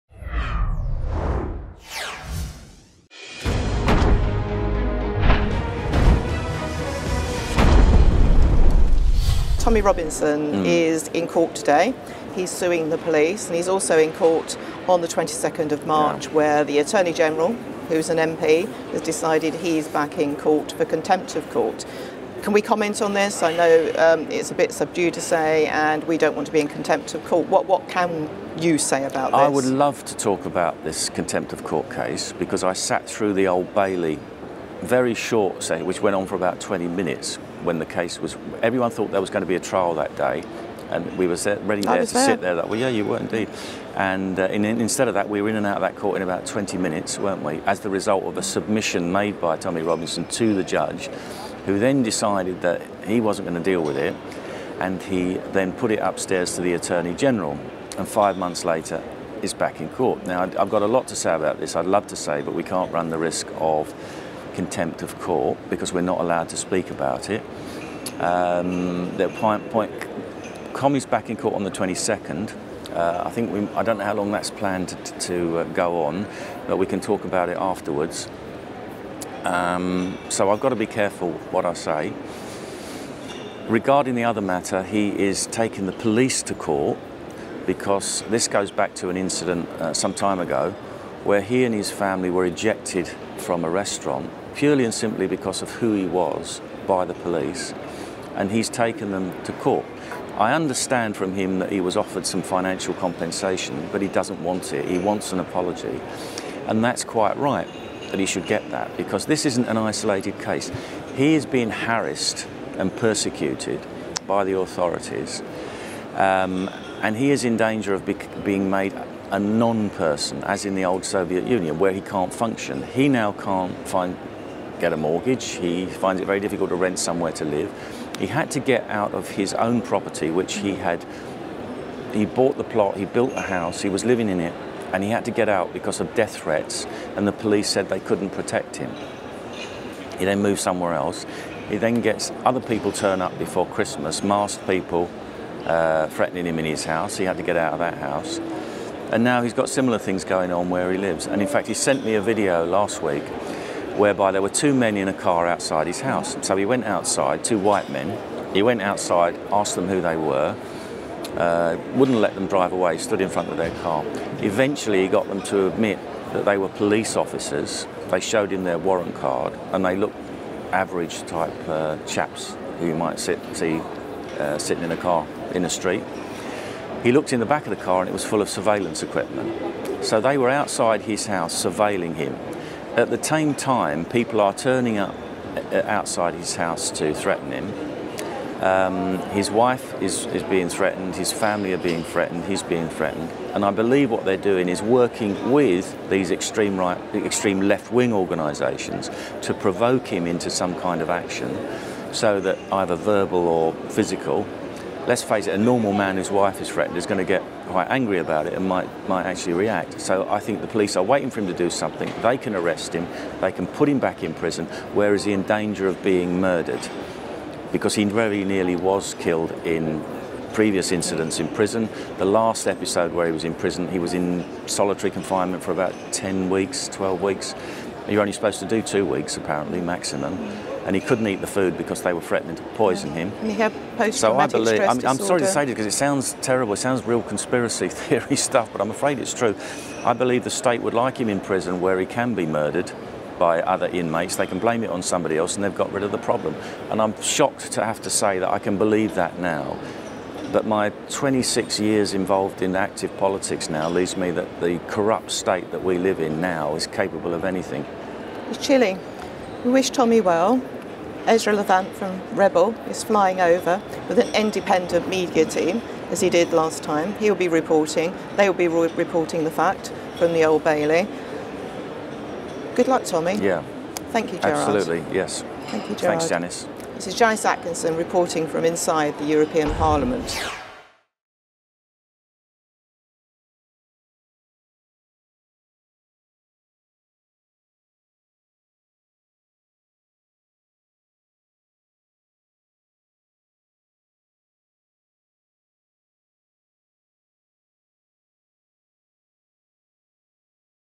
Gerard Batten talks Tommy Robinson vs. Cambridgeshire Police | Janice Atkinson
Janice Atkinson of TheRebel.media reports: I spoke with Gerard Batten, MEP and leader of UKIP about the legal troubles facing Tommy Robinson.